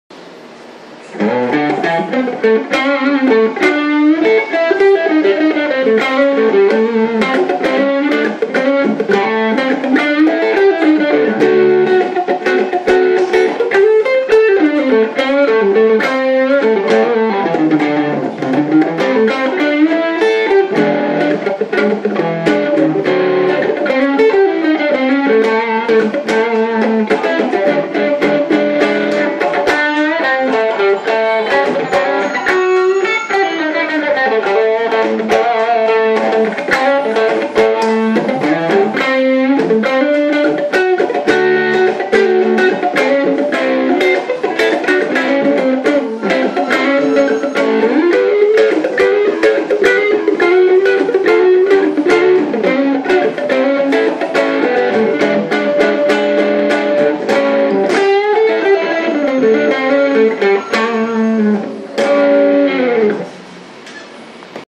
Yamaha Sg700 with ts 9 tube screamer
play 12 bars blues with Yamaha sg700 Ibanez ts9